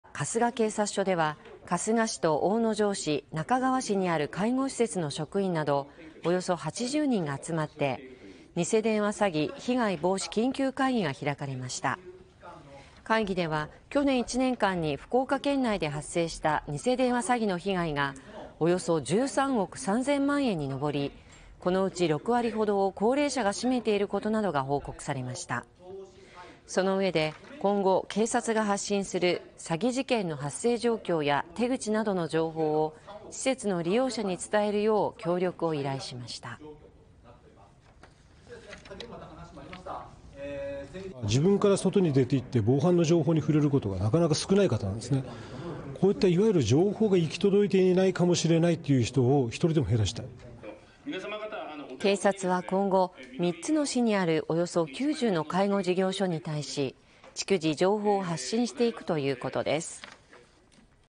被害の６割が高齢者…ニセ電話詐欺から高齢者を守るため、警察と介護施設が連携して情報発信
ニセ電話詐欺の被害に遭いやすいとされる高齢者を守ろうと、警察と介護施設の職員が集まって緊急の会議が開かれました。

◆被害の６割が高齢者…守るためには情報発信が重要
２９日午前、春日警察署で開かれた「ニセ電話詐欺被害防止緊急会議」には、春日市と大野城市、那珂川市にある介護施設の職員など約８０人が参加しました。会議では去年１年間に県内で発生したニセ電話詐欺の被害が約１３億３０００万円に上り、このうち、６割ほどを高齢者が占めていることなどが報告されました。そのうえで、今後、警察が発信するニセ電話詐欺の発生状況や手口などの情報を、施設の利用者に伝えるよう協力を依頼しました。

福岡県警春日警察署　大津良祐署長「訪問介護を受けている人は、自分から外に出て行って防犯の情報に触れることがなかなか少ない方なんですね。こういった情報が行き届いていないかもしれないという人をひとりでも減らしたい」

警察は今後、３つの市にある約９０の介護事業所に対し、逐次、情報を発信していくということです。